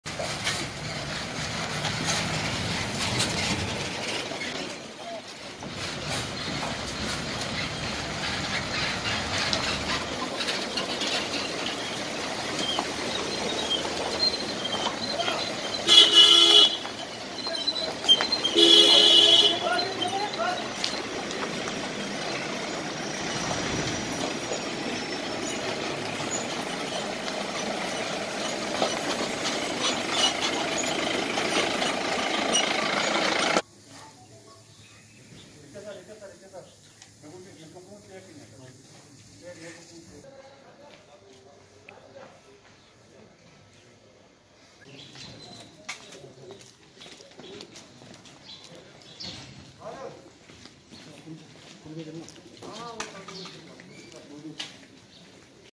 Batmalo Gunfight: 3 militants and women were killed while CRPF commandant injured in gunfight. JKNO
Batamalloo Encounter Update

Three militants and women were killed while CRPF commandant suffered bullet wounds during predawn gunfight at Batamalloo area of the Srinagar city. JKNO